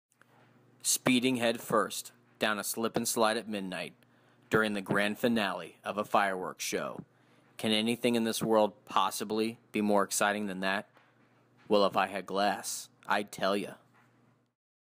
ifihadglass - Kyle Terry
ifihadglass I'd conquer marathons, jump out of an airplane, climb mountains, hit the slopes, run from zombies, and tell others all about it